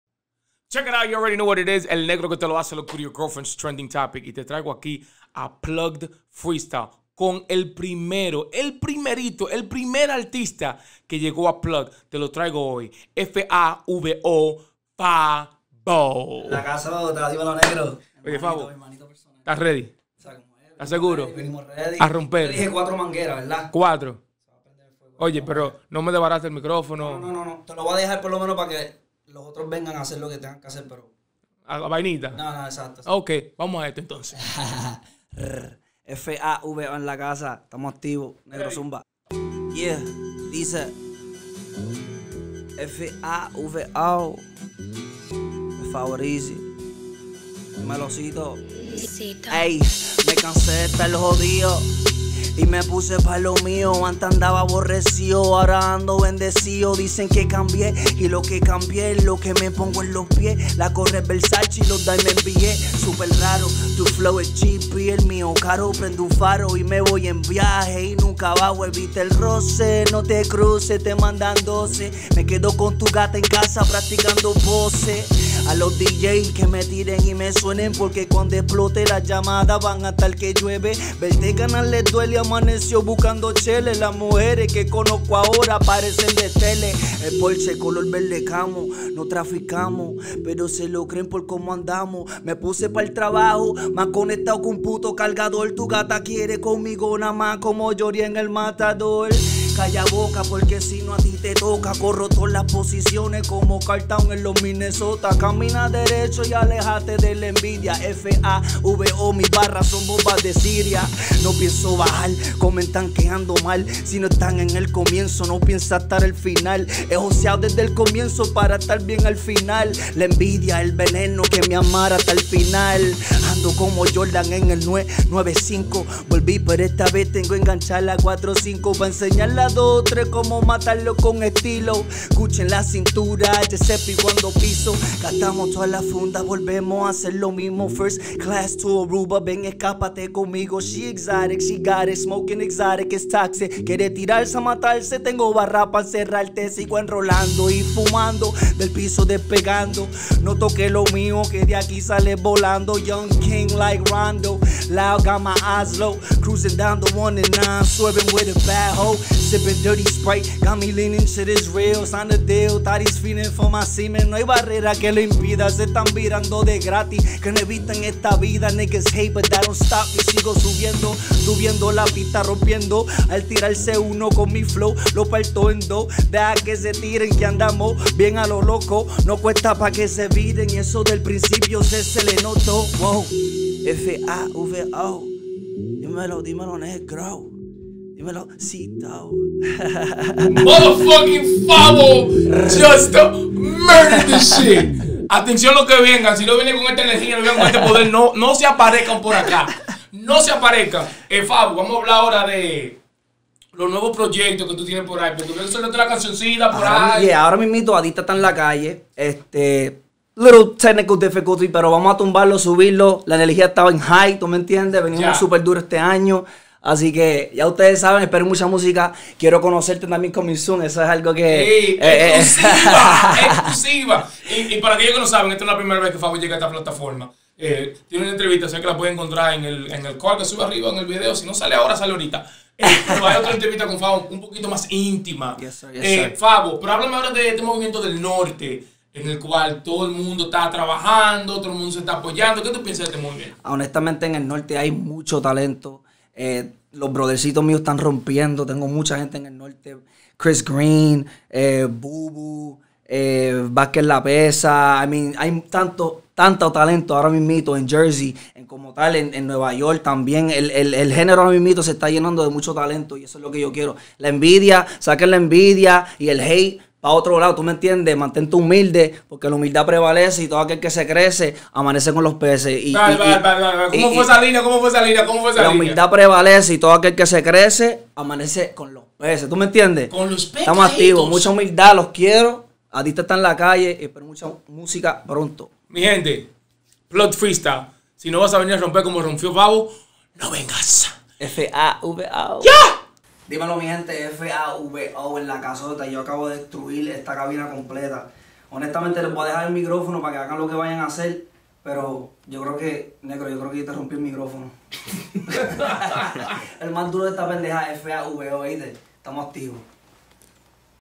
Freestyle of the week: Favo | Plugged Freestyle
This is Plugged Freestyle where artist come and show off their freestyle skills. This week we feature FAVO the first artist on our plugged segment. Una artista de la isla del encanto Puerto Rico. Dejamos saber que piensas de este freestyle de Favo en los comentarios.